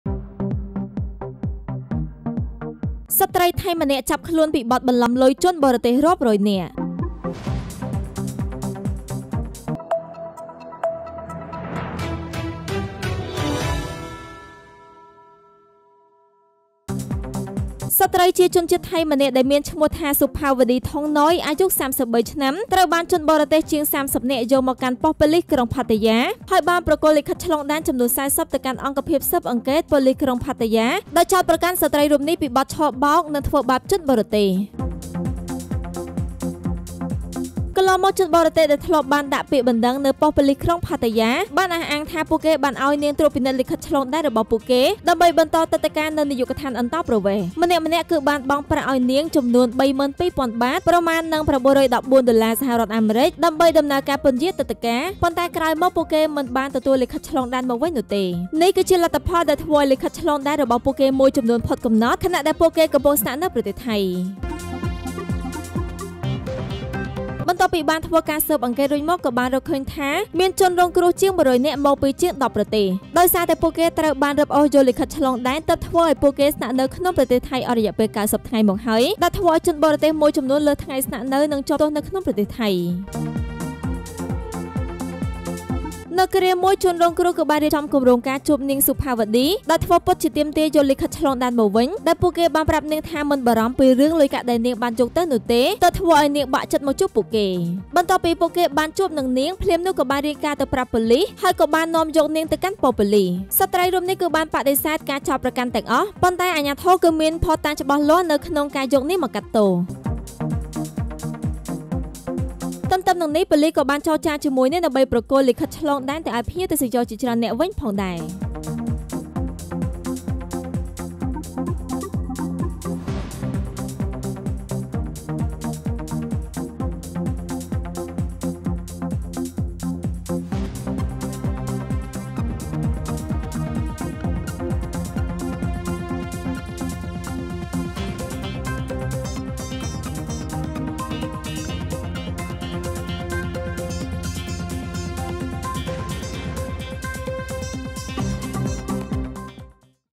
ស្រ្តីថៃម្នាក់ចាប់ខ្លួនពីបទបន្លំលុយជនបរទេសរាប់រយនាក់
ស្រ្តីជាជនជាតិថៃម្នាក់ដែលមានឈ្មោះថា Supawadee Thongnoi អាយុ៣៣ឆ្នាំ ត្រូវបានជនបរទេសជាង៣០នាក់នាំយកមកកាន់ប៉ុស្តិ៍ប៉ូលីសក្រុងប៉ាតាយ៉ា ហើយបានប្រគល់លិខិតឆ្លងដែនចំនួន ៤០ ទៅកាន់អង្គភាពស៊ើបអង្កេតប៉ូលីសក្រុងប៉ាតាយ៉ា ដោយចោតប្រកាន់ស្ត្រីរូបនេះពីបទឆបោក និងធ្វើបាបជនបរទេស។